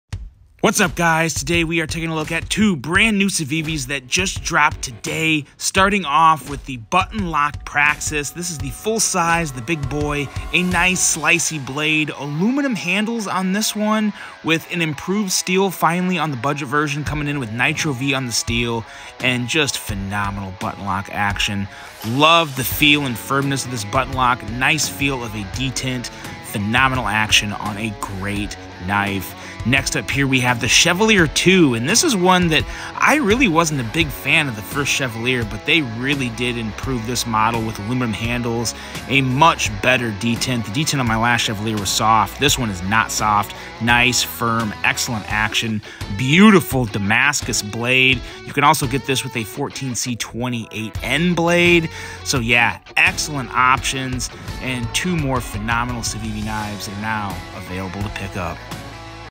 2 AMAZING BRAND NEW CIVIVI KNIVES!! 🔥🔥😮‍💨
This short takes a look at two brand new Civivi knives!